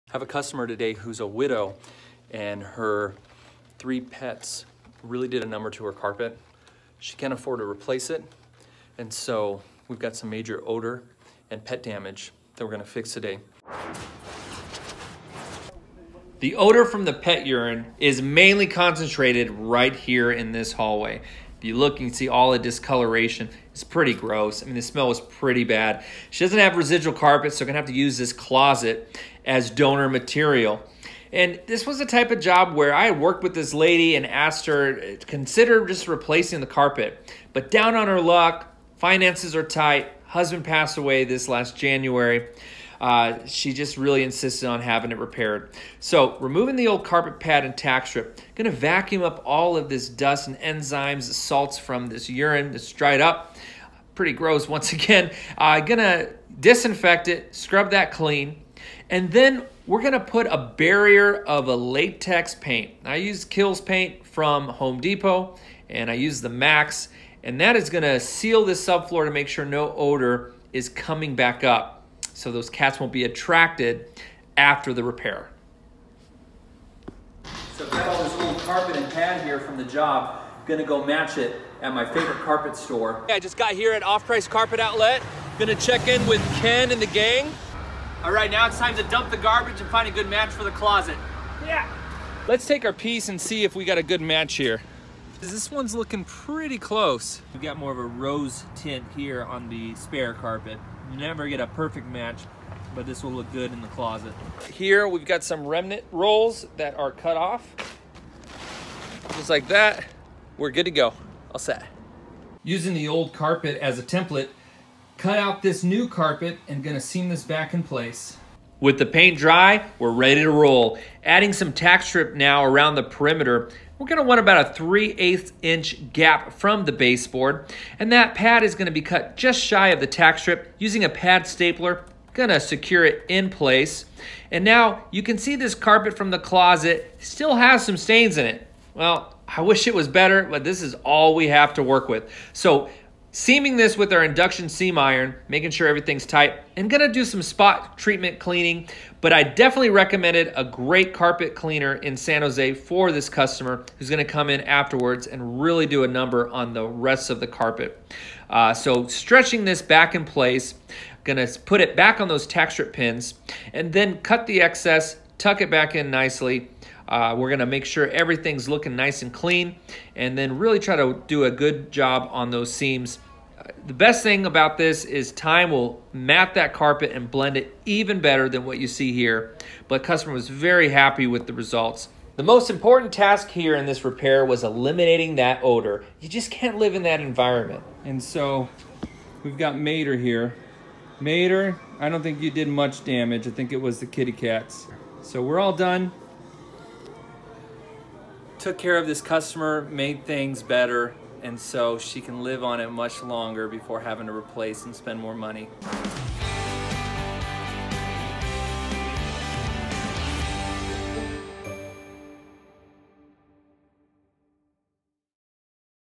Extreme Pet Odor in Carpet!!!
Josh explains how to repair a very damaged piece of carpet in a hallway.

if you are in need of carpet repair.